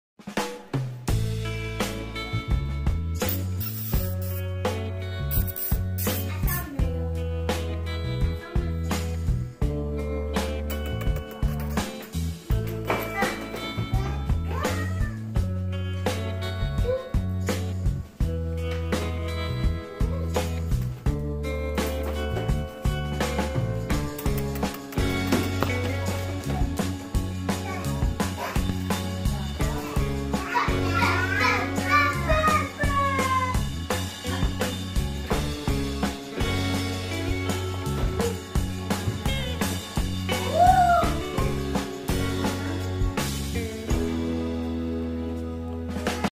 Mini goldendoodles 8 weeks old
Chilling in their holding pen while waiting for the Vet 😀